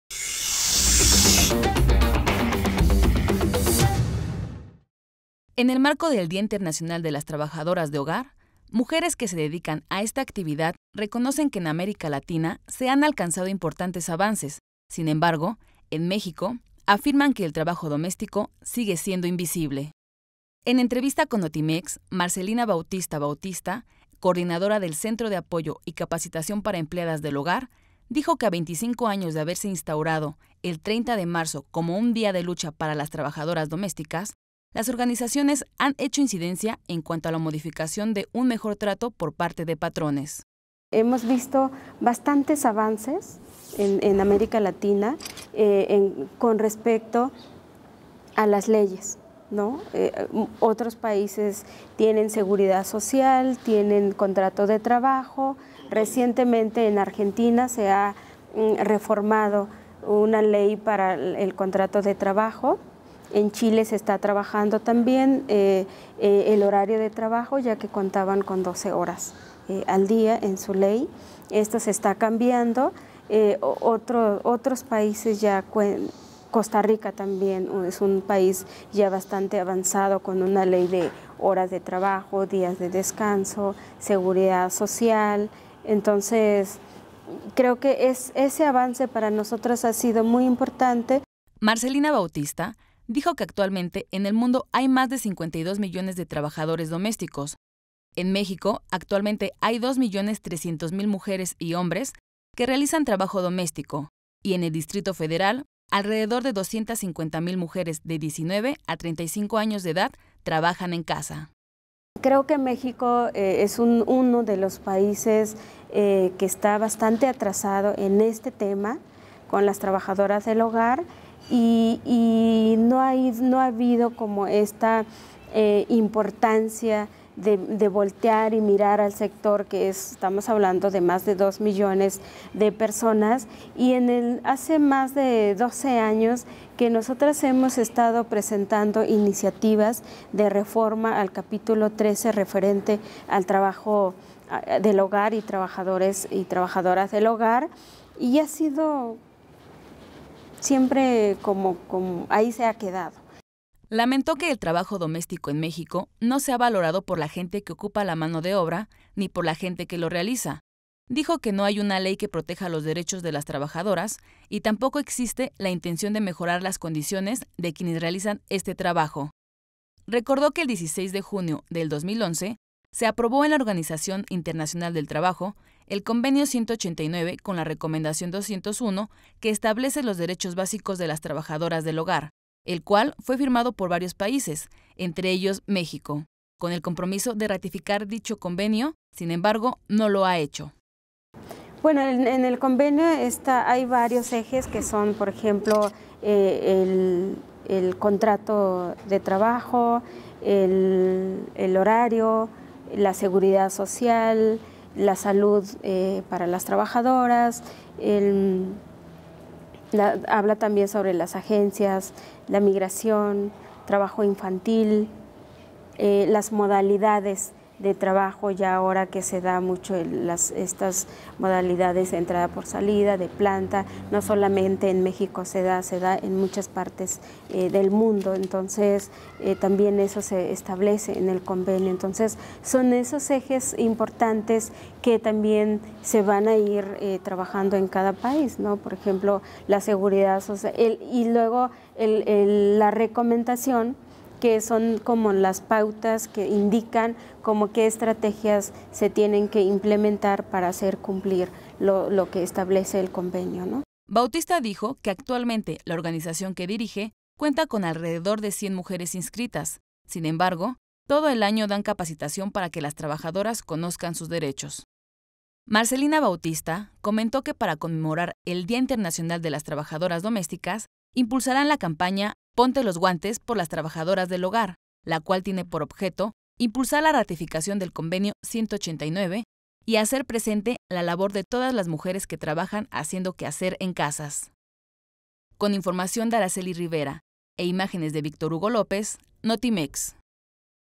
Día Internacional de las Trabajadoras de Hogar
"Ponte los guantes por las trabajadoras del hogar" campaña de concientización